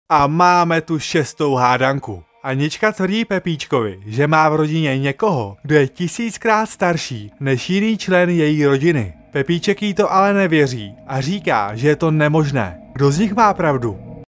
Hádanka 6: Může být někdo tisíckrát starší?🕵️
Anička Pepíčkovi tvrdí, že má v rodině někoho, kdo je tisíckrát starší než jiný člen z její rodiny. Pepíček si myslí, že je to nemožné. Kdo má pravdu?